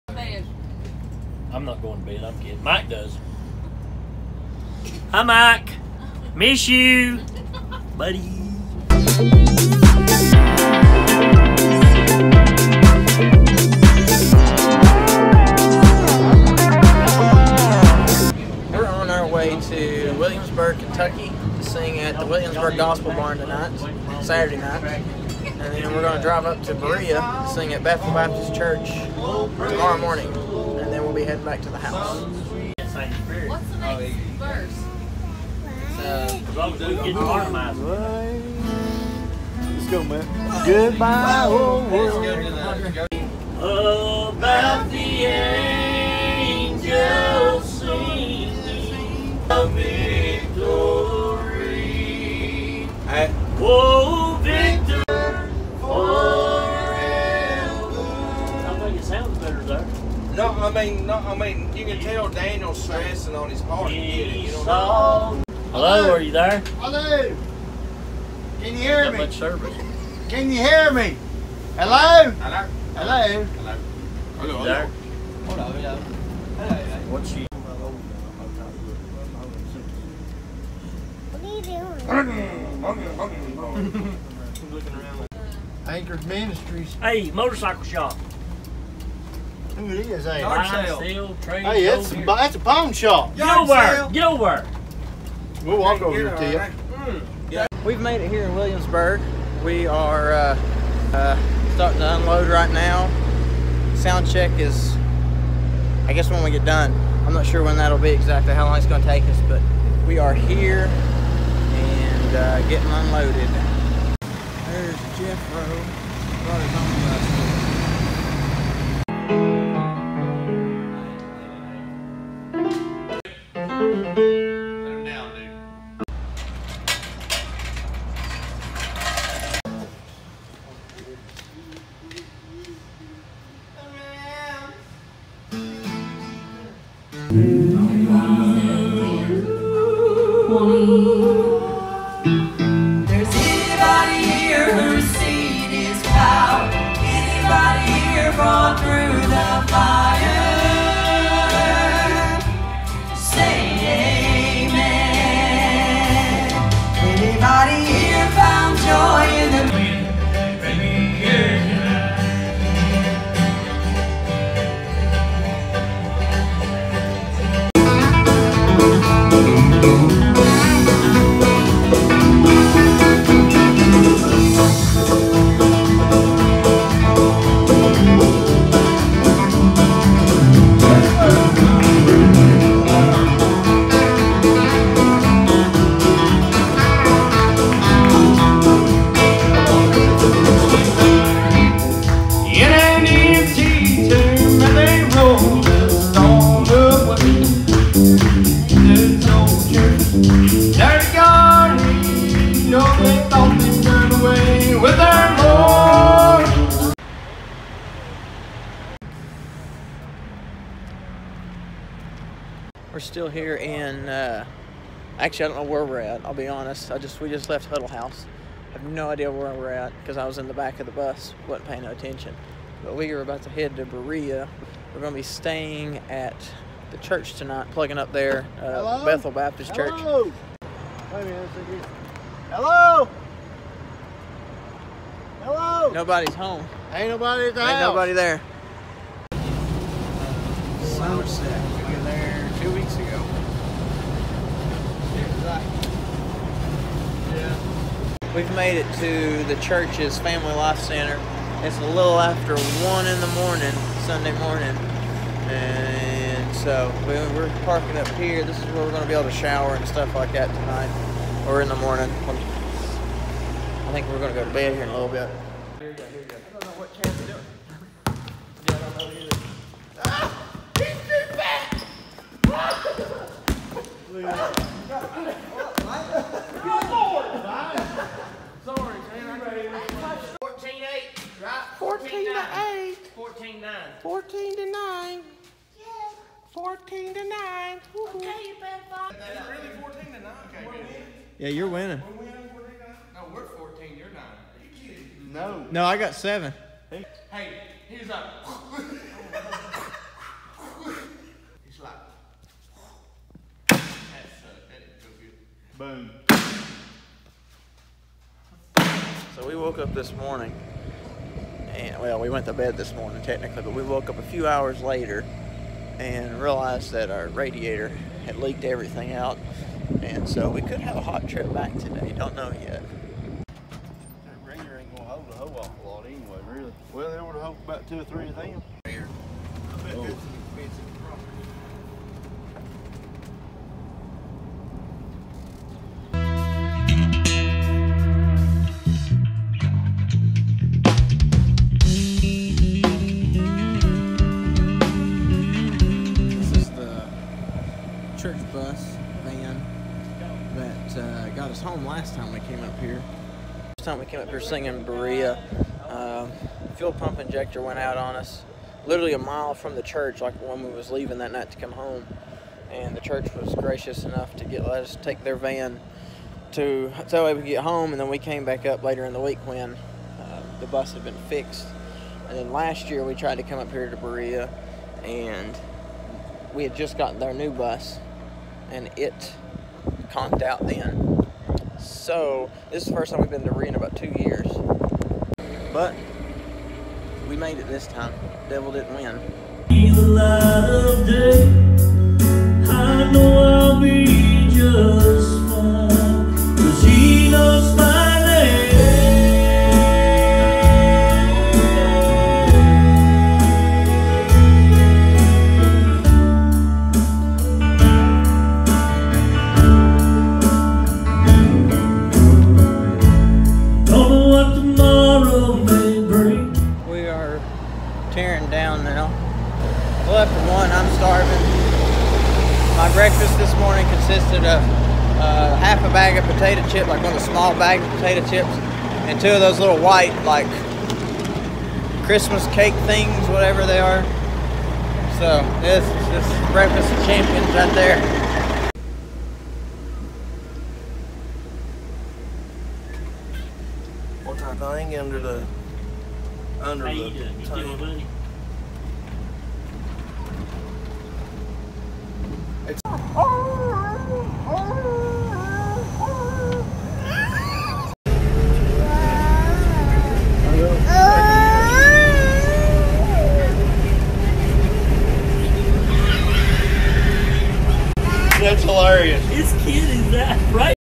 Tour Vlog 4 - Williamsburg / Berea, KY
Chris Holder and Journey Home sings at the Williamsburg Gospel Barn and Bethel Baptist Church.